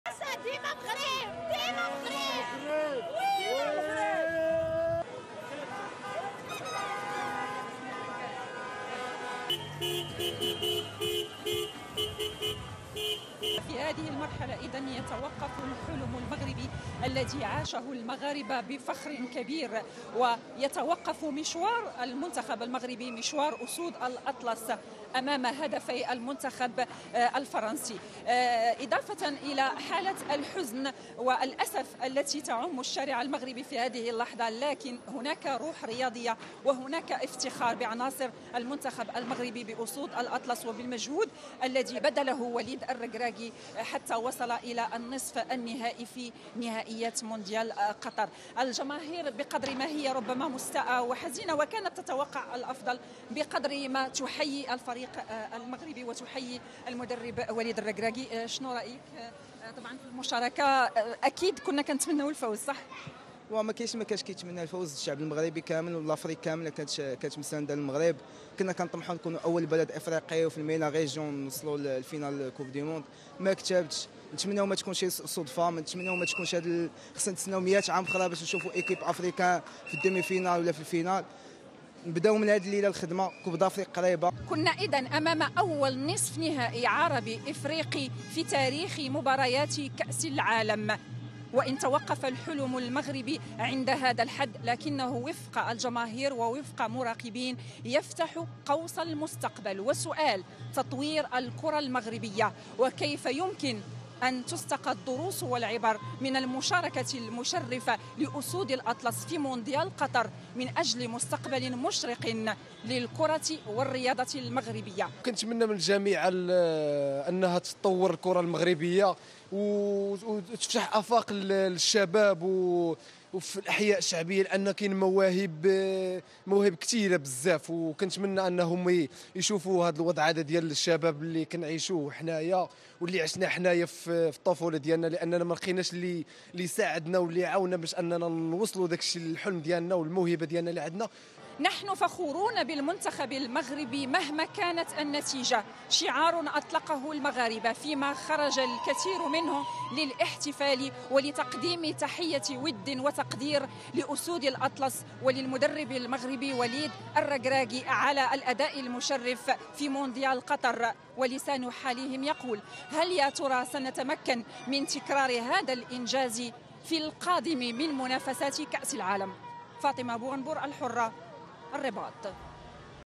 ردود فعل متباينة في المغرب بعد خسارة المنتخب أمام فرنسا
ينت ردود فعل الجماهير المغربية إزاء خسارة المنتخب المغربي أمام فرنسا في مباراة نصف نهائي كأس العالم لكرة القدم في قطر.

حمل تطبيق "الحرة كونيكت" للتلفزيونات والهواتف الذكية:

أمازون فاير